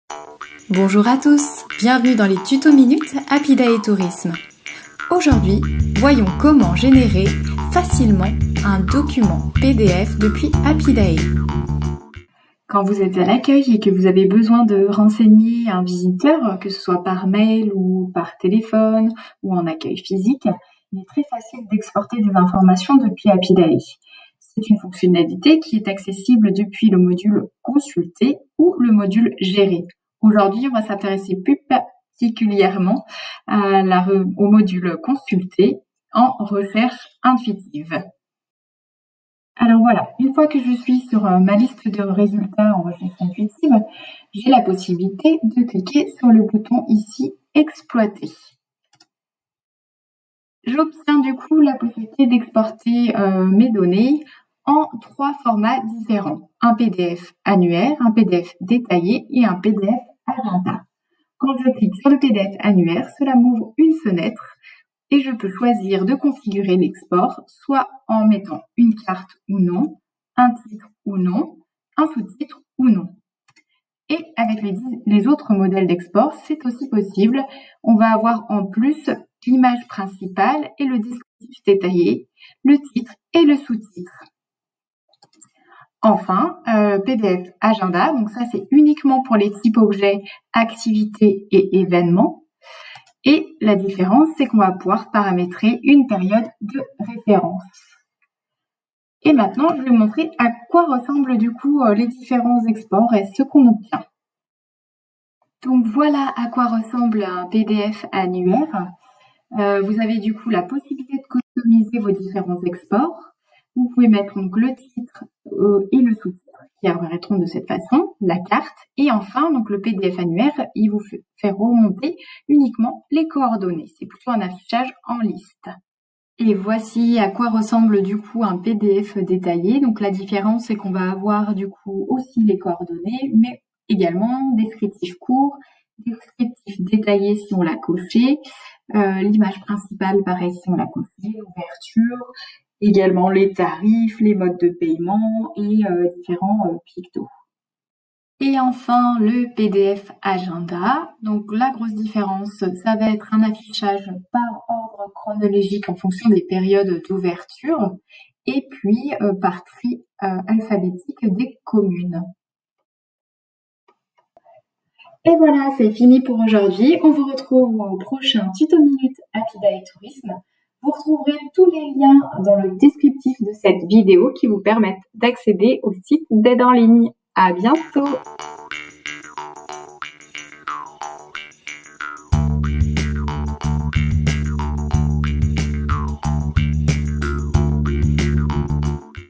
Comment générer facilement votre documentation ? (export pdf) - un Tuto Minute Apidae Tourisme
Quand vous êtes à l'accueil et que vous avez besoin de renseigner un visiteur, il est très facile d'exporter des informations depuis Apidae Tourisme pour lui imprimer ou lui envoyer par mail.

Pour accéder à l'ensemble des tutos de l'aide en ligne, c'est par